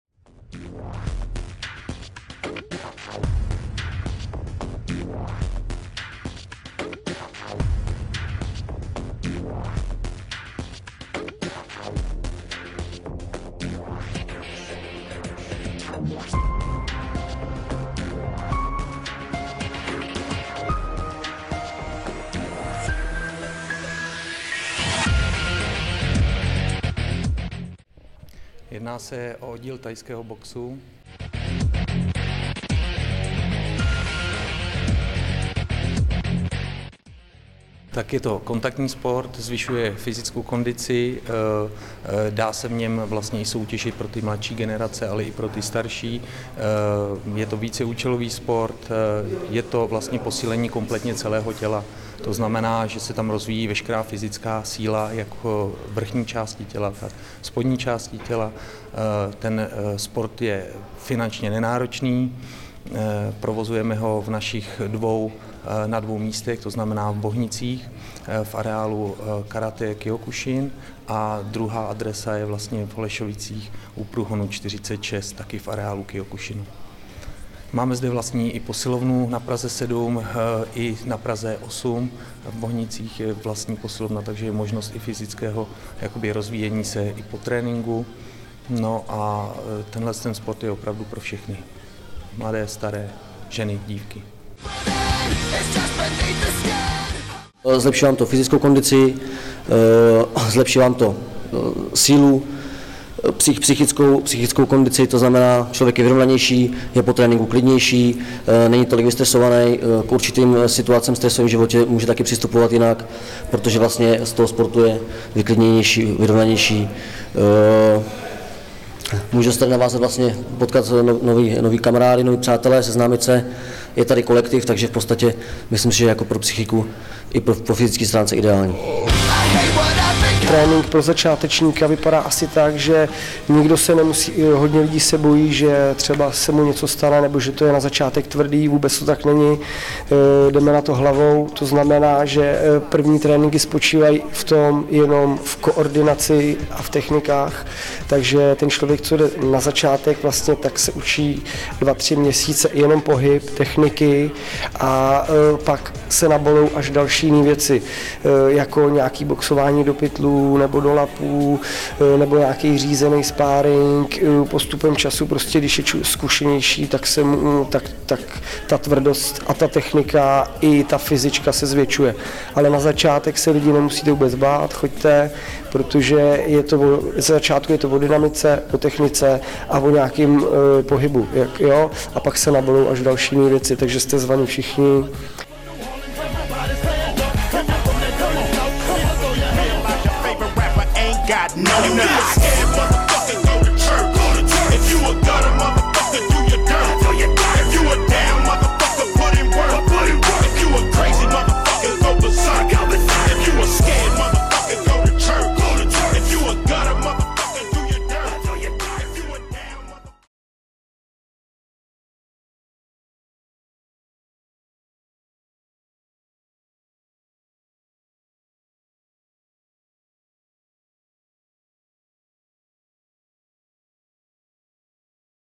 Představujeme sportovní kluby -RSF Praha-
TV Praha 8 představuje sportovní klub RSF Praha Muay Thai Klub se sídlem na Praze 8.